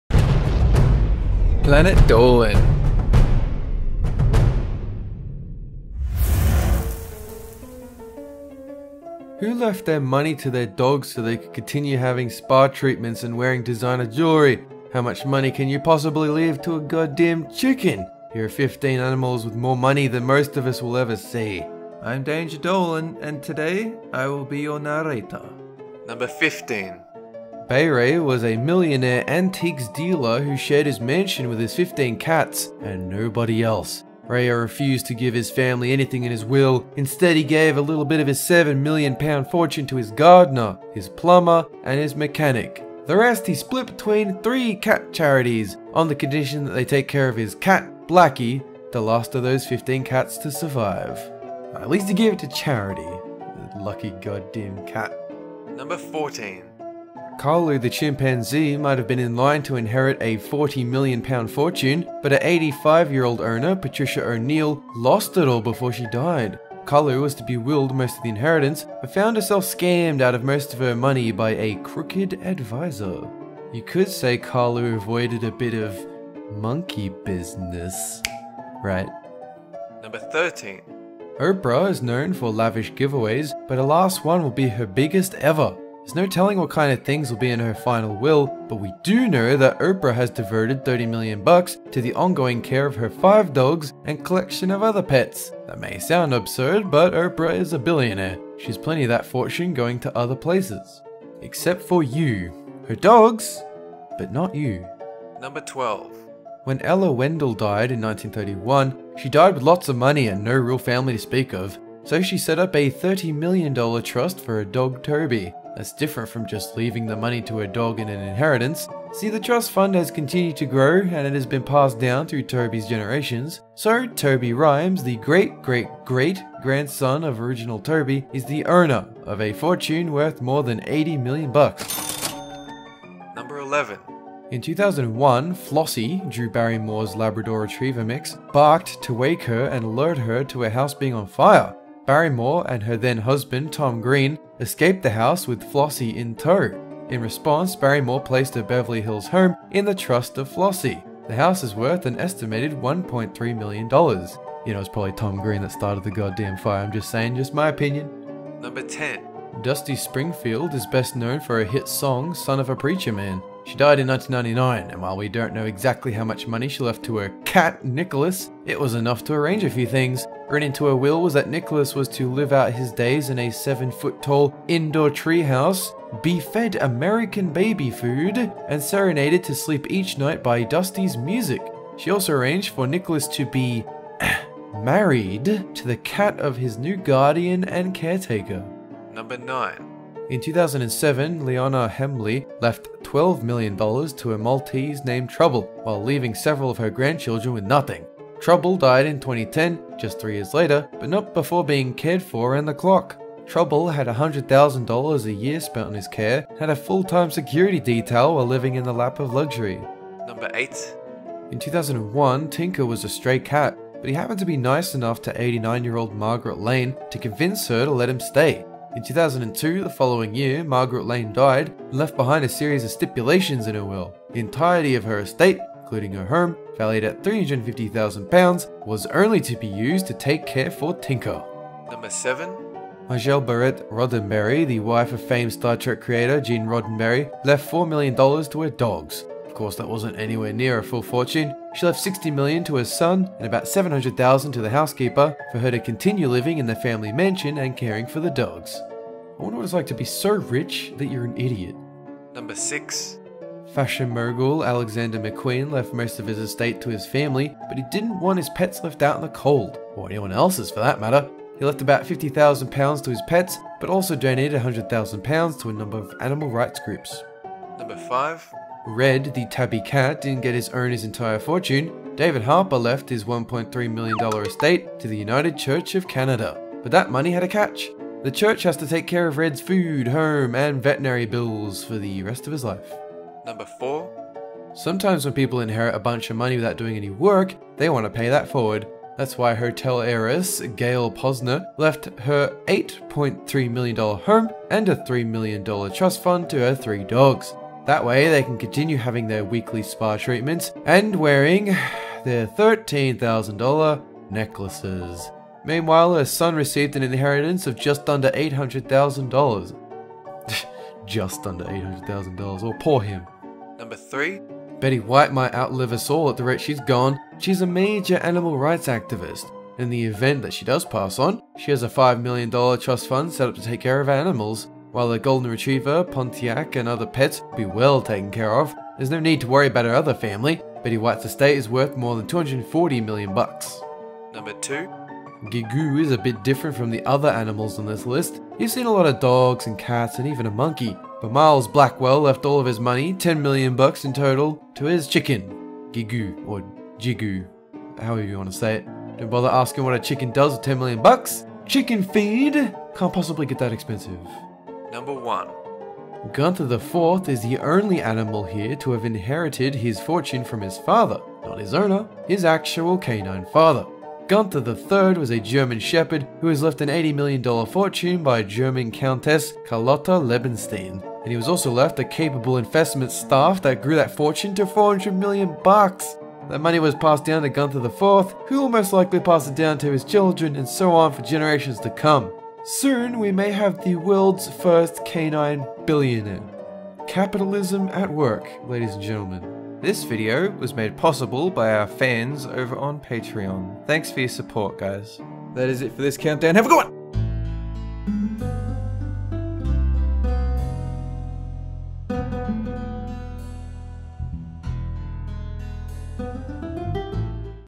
15 Animals That Inherited MILLIONS
Who left their money to their dogs so that could continue have spa treatments and wearing designer jewelry? How much money can you possibly leave to a chicken? Here are 15 animals with more money than most of us will ever see